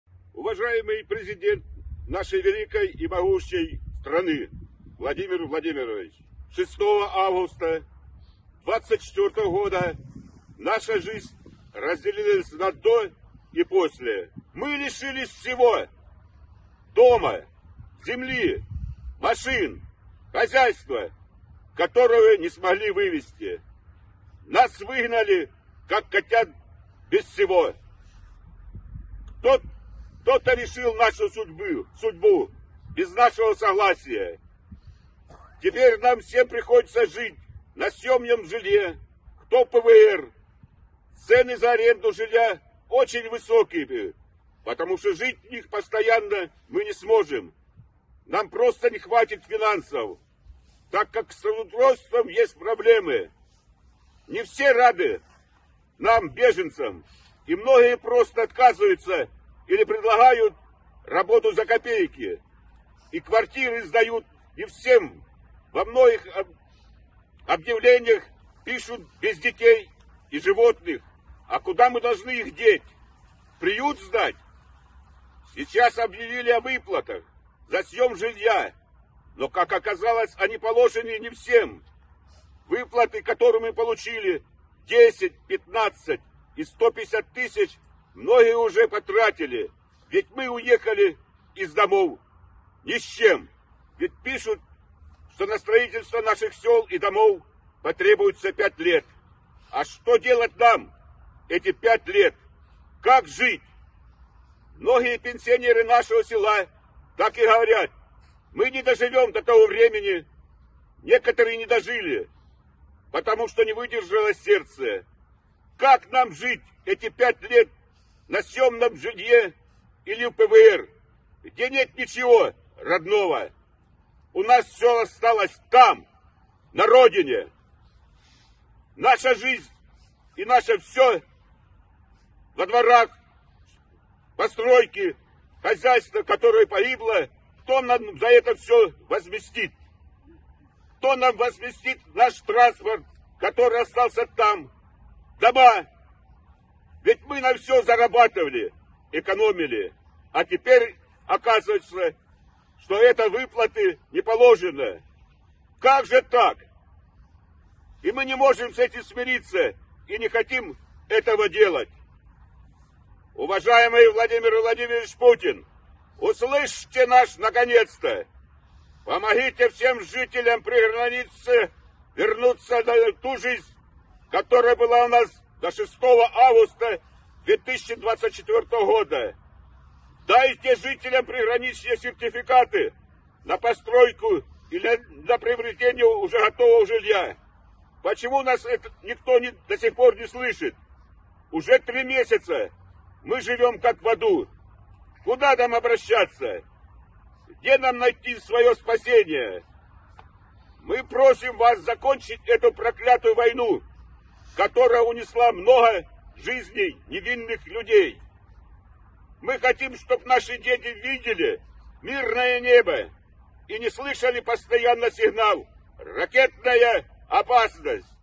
🟠Жители Курской области попросили Путина остановить войну
Более 40 жителей села Ольговка Курской области записали видеообращение к Путину с просьбой прекратить войну. Люди рассказали, что их село разгромлено, возвращаться туда опасно, снимать жильё многим из них не по карману, а выплаты от государства уже закончились.

Местные власти не объявили об эвакуации, поэтому некоторые ольговчане погибли или пропали без вести, не успев покинуть населённый пункт. Кто-то решил, что обстрелы закончатся через несколько дней, и тоже остался дома. Теперь о судьбе этих людей ничего не известно. По словам чиновников, на восстановление Ольговки уйдёт около пяти лет. Пенсионеры опасаются, что не доживут до этого.

«Мы три месяца как в аду. Куда нам обращаться, где найти спасение? Просим вас закончить эту проклятую войну, которая унесла много невинных жизней. Мы хотим, чтобы наши дети видели мирное небо, не слушали сигнал ракетной опасности. Разберитесь в этой ситуации и услышьте жителей приграничья. Не по своей воле мы всё это терпим»,
 — заявили жители.

🟠 Предложите новость @ActivaticaBot